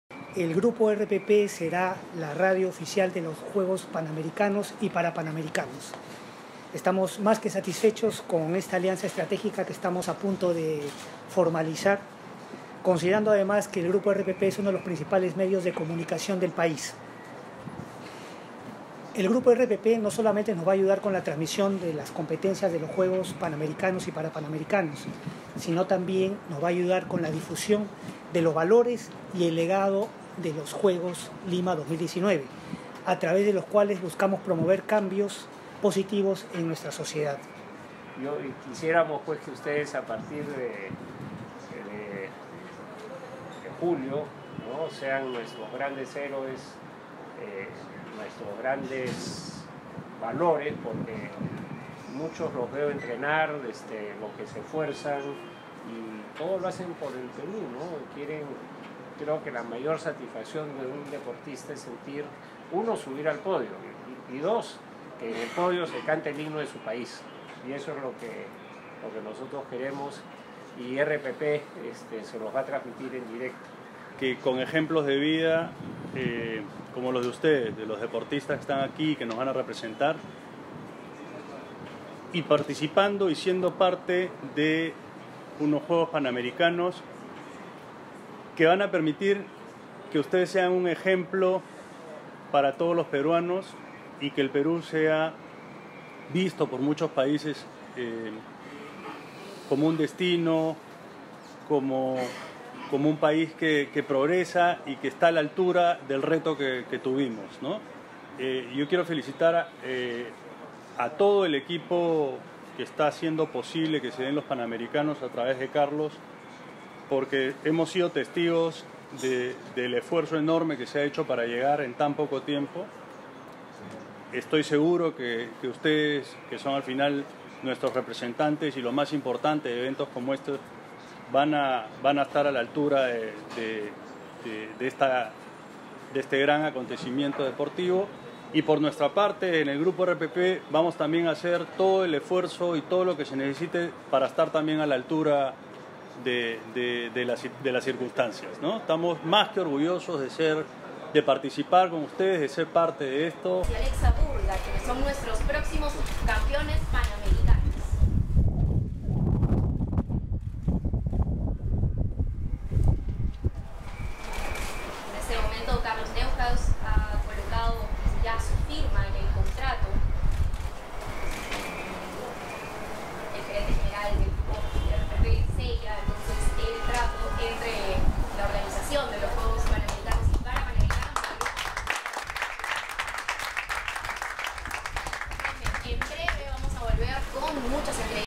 RPP transmitira los Juegos de Lima 2019
Lima 2019 firmó una alianza estratégica con el Grupo RPP, con la finalidad de difundir y promocionar los Juegos Panamericanos y Parapanamericanos de Lima 2019.
Se dedicarán 955 horas para la transmisión y difusión de los Juegos.
• Se emitirán alertas informativas en las siete radios del Grupo RPP, cada vez que un peruano obtenga una medalla y se bata algún récord deportivo. Fuente: Prensa Lima 2019.
Más información ingresando al blog "Hola Es Un Gusto" en el siguiente enlace.